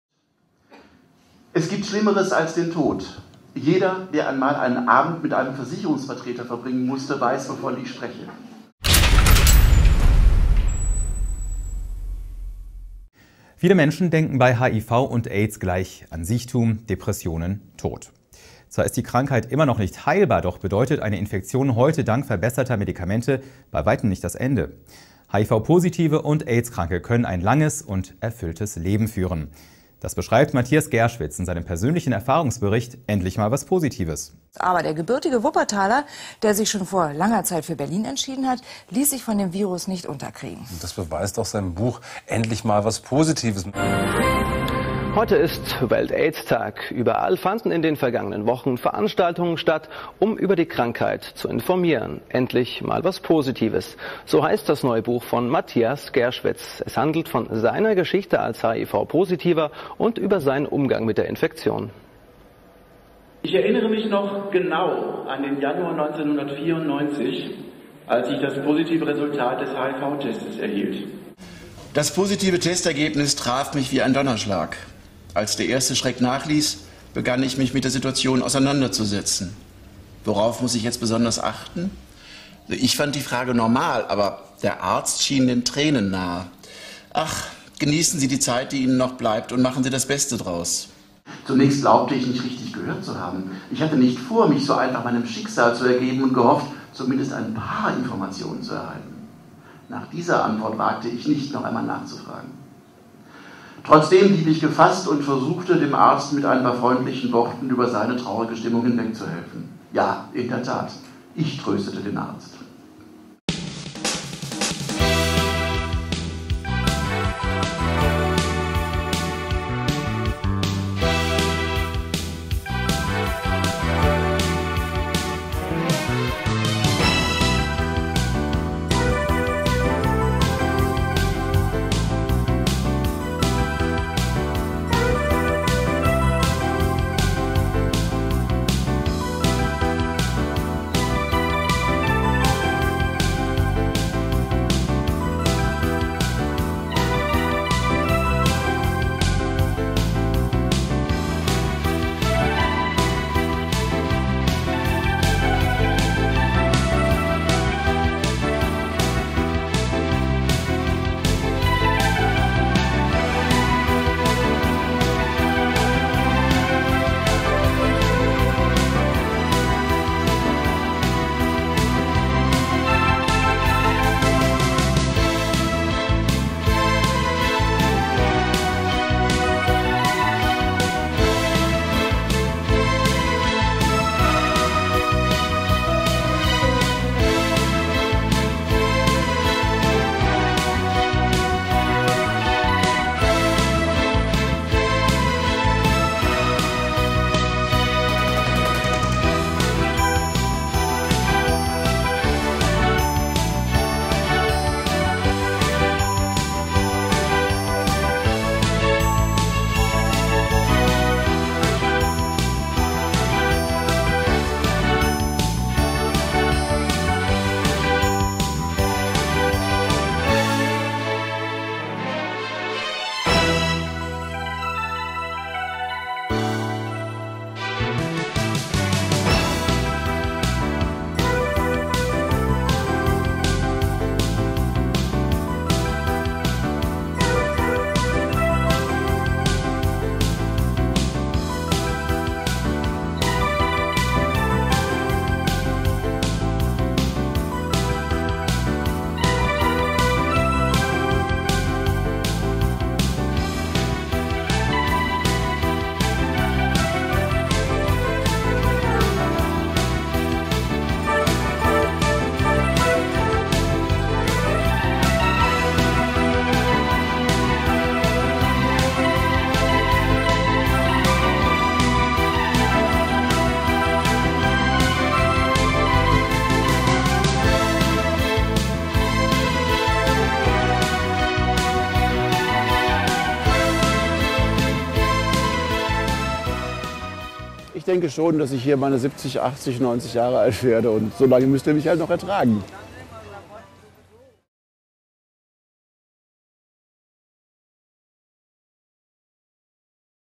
Endlich mal was Positives: Matthias Gerschwitz informiert über HIV
Es gibt viele Bücher zu HIV und AIDS, aber nur wenige, die einen direkten Einblick in das Leben mit dem Virus geben. In »Endlich mal was Positives« beschreibt Matthias Gerschwitz, der 1994 das Testergebnis HIV positiv erhielt, seinen Umgang mit der Infektion ohne Larmoyanz oder Betroffenheitspathos, sondern optimistisch und zuweilen auch etwas provokativ.

Das Buch soll Mut machen und informieren: Es wendet sich nicht nur an Positive, insbesondere Neu-Infizierte und ihre Freunde und Verwandte, die sich plötzlich auf eine veränderte Lebenssituation einstellen müssen, sondern auch (und besonders) an diejenigen, die bislang keine Notwendigkeit sahen, sich mit der Infektion zu befassen, weil sie immer noch glauben, sie wären von HIV nicht betroffen. Eine trügerische Einstellung, denn es kann jeden treffen, egal, ob Mann oder Frau, ob homo-, bi- oder heterosexuell. Das Virus ist nicht wählerisch.

Endlich mal was Positives ist teils Erfahrungsbericht, teils Statement. Das Buch zeigt, dass man auch mit HIV das Lachen nicht verlernen muss und mit einer unheilbaren Krankheit zukunftsorientiert leben kann. Und das ist doch endlich mal was Positives.

Matthias Gerschwitz wurde 1959 in Wuppertal geboren und wuchs in Solingen auf. Nach Abitur und Bundeswehr folgte das Studium der Werbewirtschaft in Pforzheim (1982 bis 1984), das er in Frankfurt am Main fortsetzte und 1987 als Kommunikationswirt abschloss. Von 1984 bis 1991 arbeitete Gerschwitz im Marketing eines mittelständischen Markenartikelunternehmens (delta pronatura) im Raum Frankfurt, von 1991 bis 1992 war er als Product Manager Functional Products bei Firmenich (ein weltweit operierender Konzern der Riechstoff- und Aromenindustrie) in der Nähe von Köln tätig. 1992 machte er sich mit einer Werbeagentur in Berlin selbständig.

Seit 2007 schreibt Gerschwitz auch Bücher, vorwiegend Chroniken. So entstand die Geschichte des 1827 erfundenen und damit eines der ältesten Markenartikel Deutschlands: »Bullrich-Salz: Marke · Mythos · Magensäure« und die Chronik der ältesten Kneipe Berlin-Charlottenburgs (Wilhelm Hoeck 1892): »Molle und Medaille«. Im Januar 2010 erschien »Das Haus in der Kaiserstraße«, ein Buch über das 1831 erbaute Haus in Solingen-Wald, in dem der Autor von 1959 bis 1981 lebte. Im Frühjahr 2011 erschien als Auftragswerk die Biographie des böhmischen Unternehmers Johann Schicht (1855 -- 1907) unter dem Titel »Der große Aussiger«. Im November 2011 folgte ein Bildband/Chronik-Kombination über die evangelische Kirche Solingen-Wald: »Mittendrin -- Die evangelische Kirche Solingen-Wald«.

Mit »Endlich mal was Positives« legte Gerschwitz 2009 eine Chronik der besonderen Art vor: die Geschichte seiner 1994 festgestellten HIV-Infektion und wie er damit umgeht. Er erzählt aus seinem Leben und lässt den Leser an seinen Gedanken und Meinungen teilhaben; so entsteht das Bild eines optimistischen und fröhlichen Menschen, der sich von ein paar Viren nicht unterkriegen lässt. Und der eindeutig für "Safer Sex" plädiert.

Matthias Gerschwitz ist Botschafter des Welt-Aids-Tags und Protagonist der Welt-Aids-Tag-Kampagne 2010. Er wurde für Endlich mal was Positives mit dem Annemarie-Madison-Preises 2010 ausgezeichnet. 2011 war das Buch in der Vorauswahl um den Medienpreis der Deutschen AIDS-Stiftung.

Matthias Gerschwitz
Endlich mal was Positives
Paperback, 96 Seiten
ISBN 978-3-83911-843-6
€ 9,95